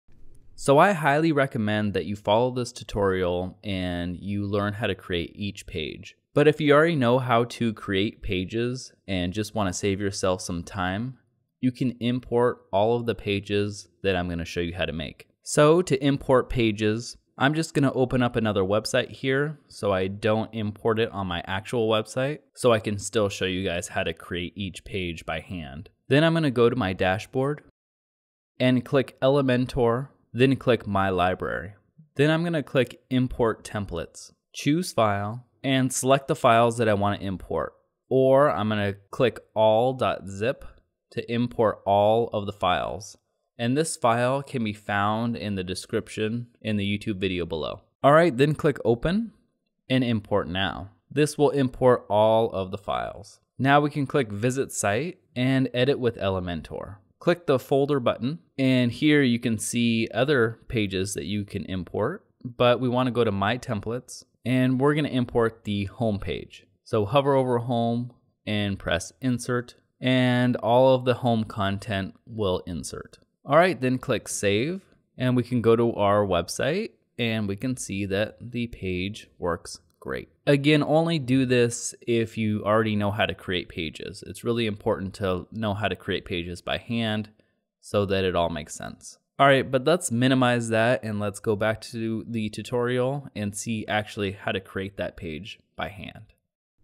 Import Elementor Pages In WordPress #18
Learn how to import templates in Elementor to save your self lots of time and make your website look pro.